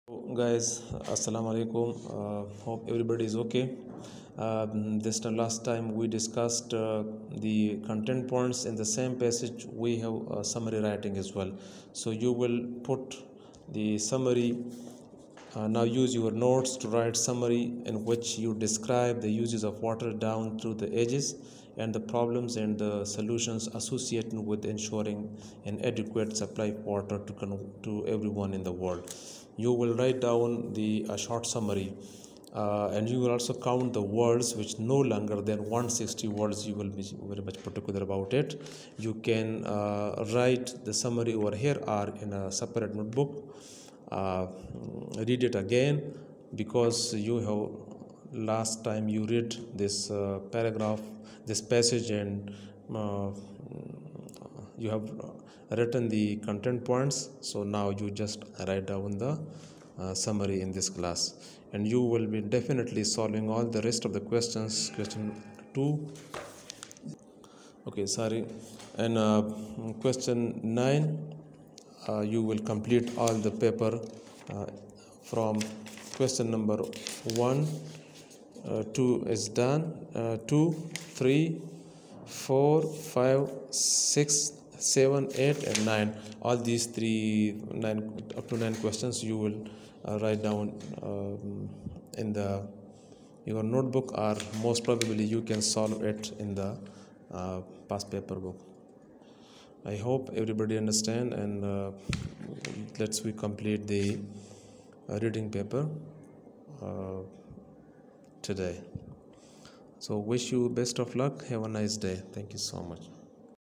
Lecture 4 Summary writing paper 2, section 1 part b
This video is writing summary in GCE o level examination